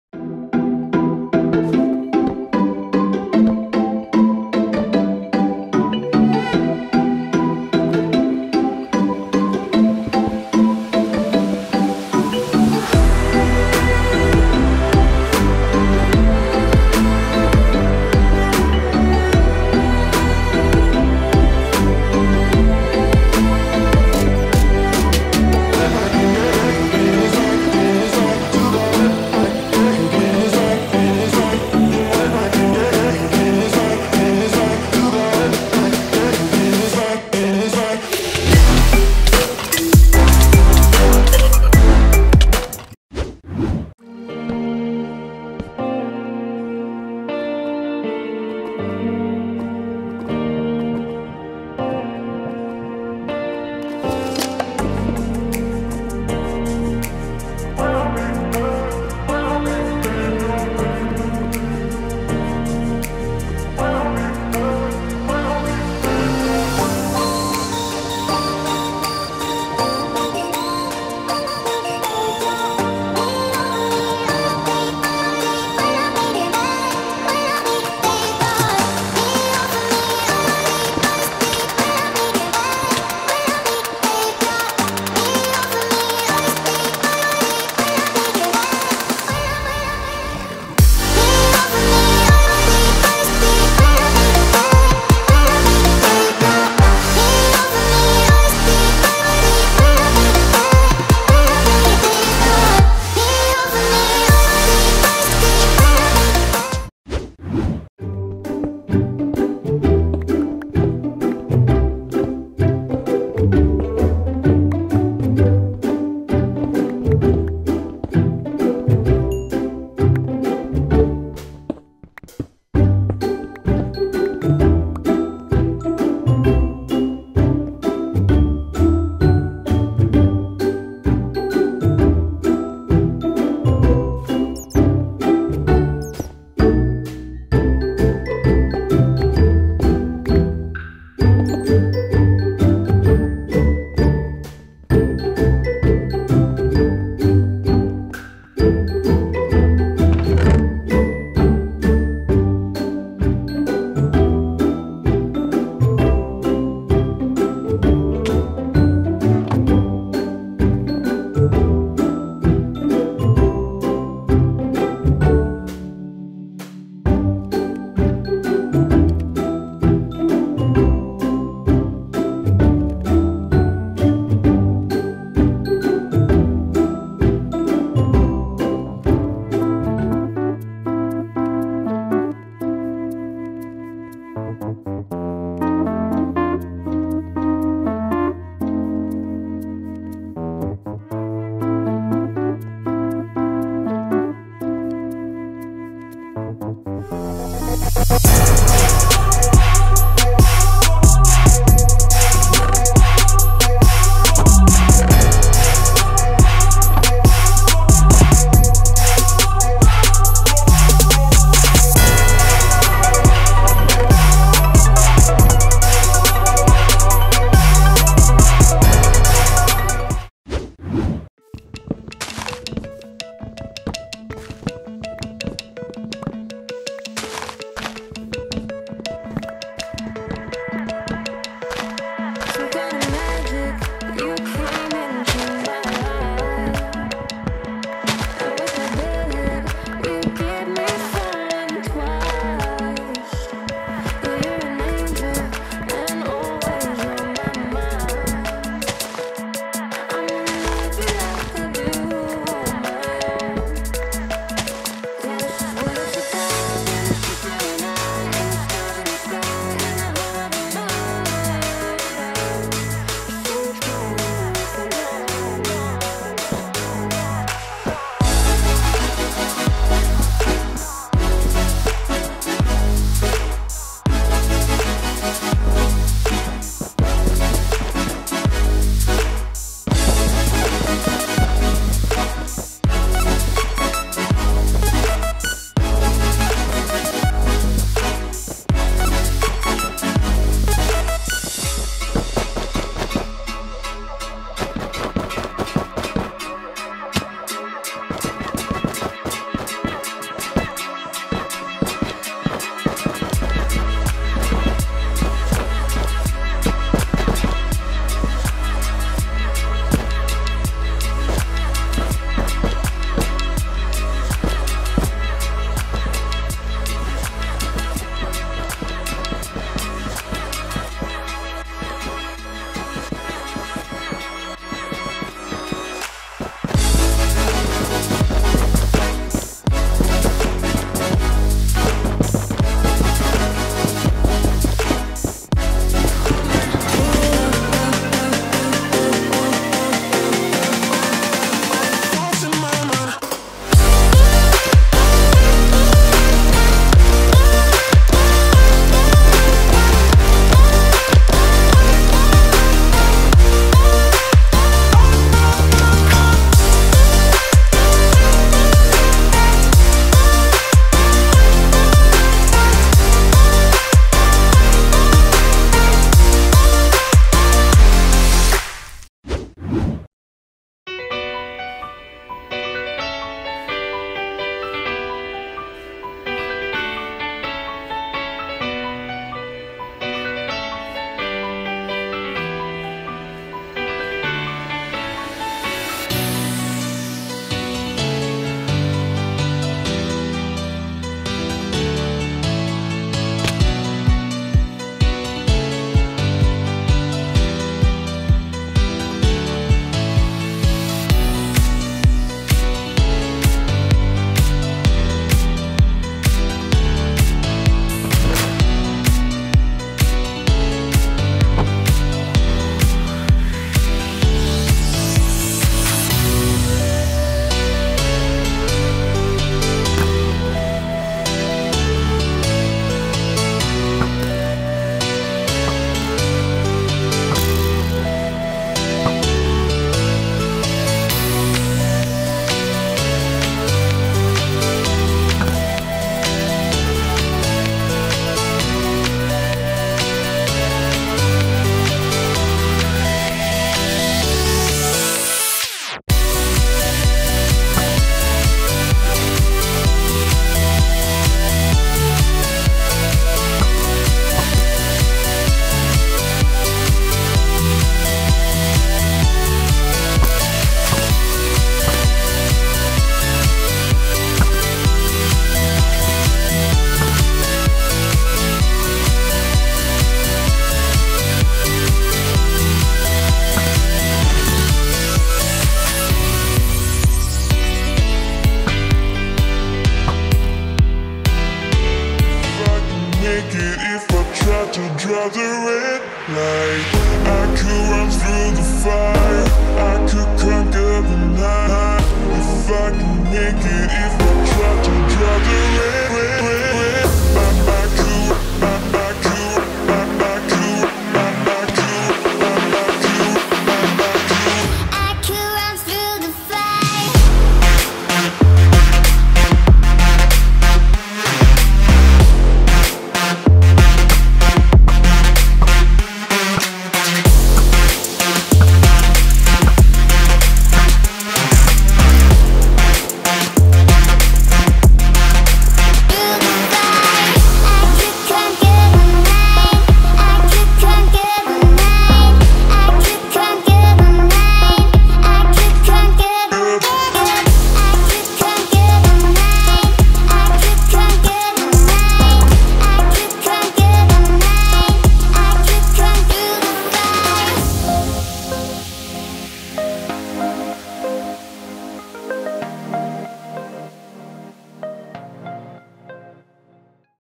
Herobrine wants to kill me!!!! Heeeeelp....
I've recorded all these memes for myself!
Hopefully you'll enjoy this compilation❤️